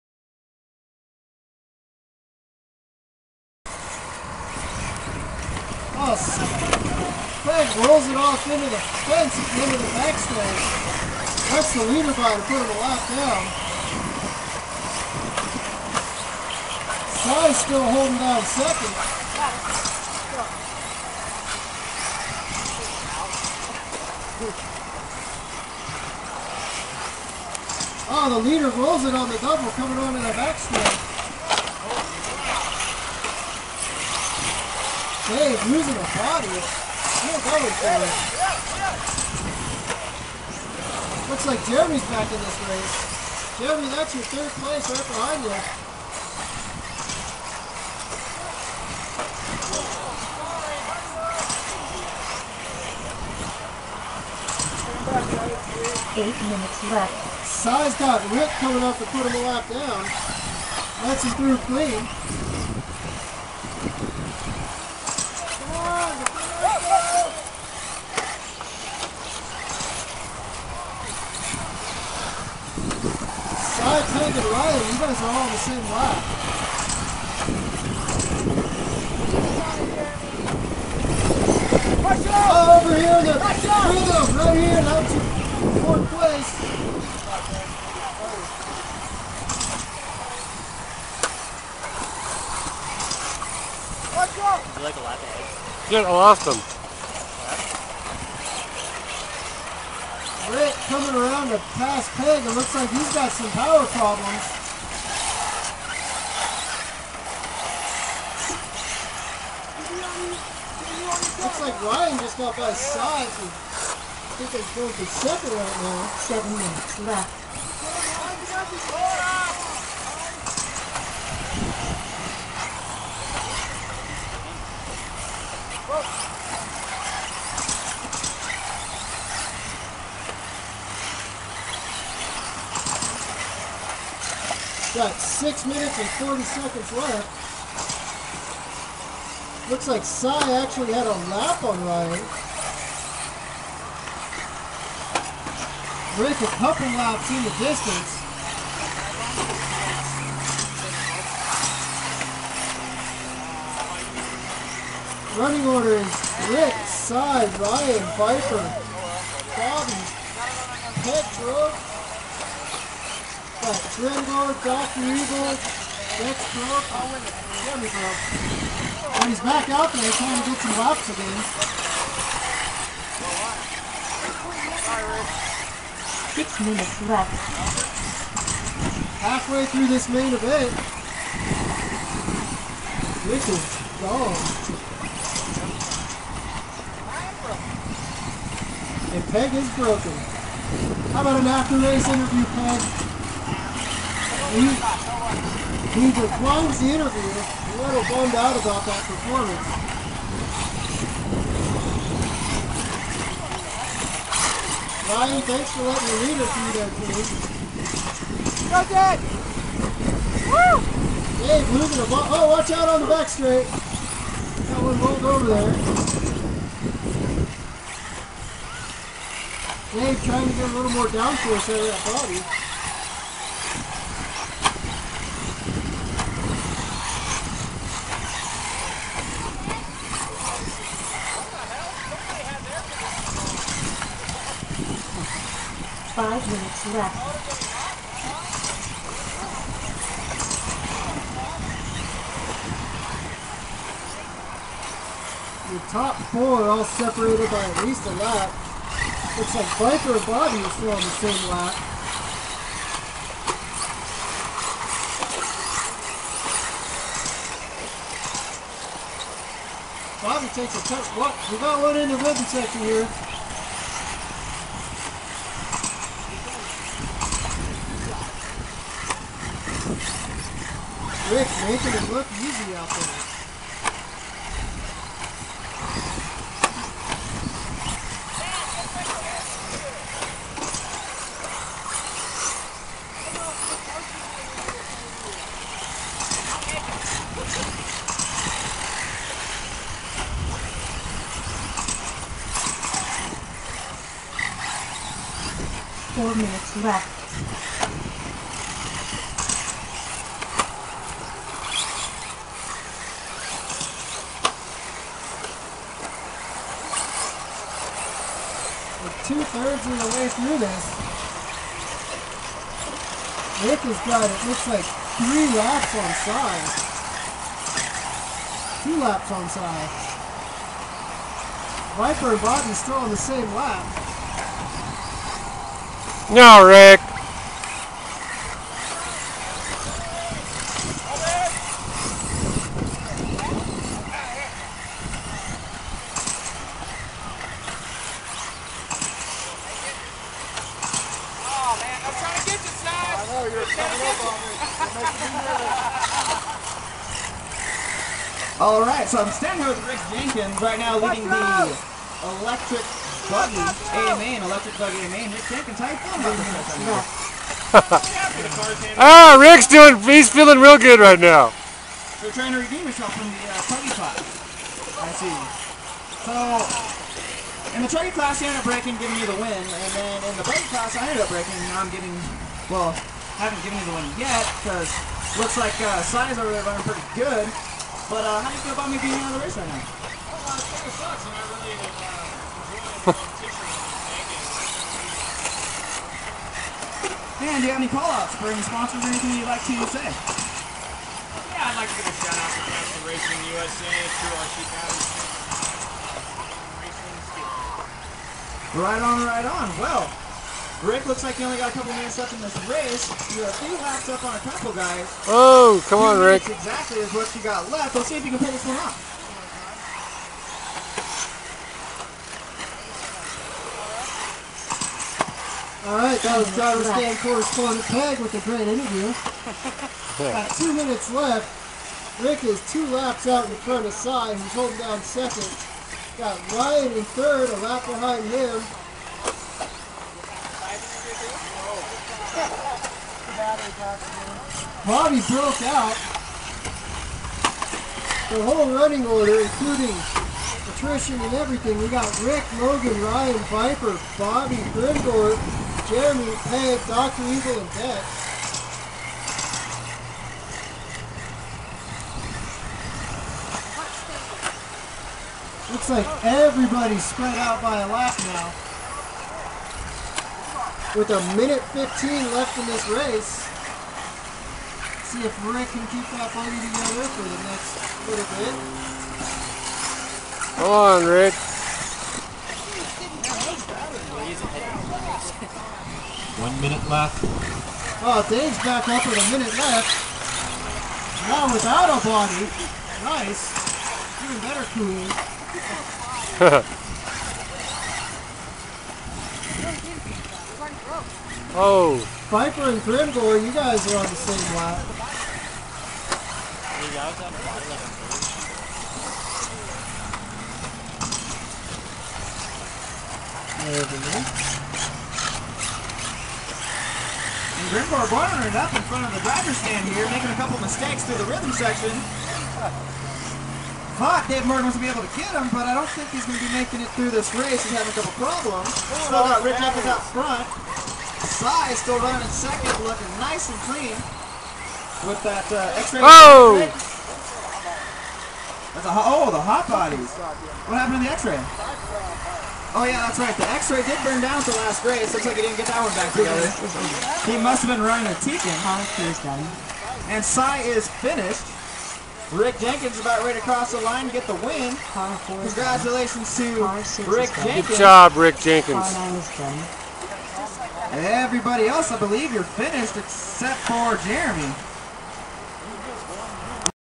E-Buggy A main Reno, NV 5-16-10
Caster Racing Rick Jenkins with his caster fusion pro 1.5R buggy